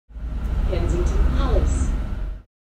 Kensington Palace
iBus announcement recorded on route 23 (DNH39127) on the 05/12/18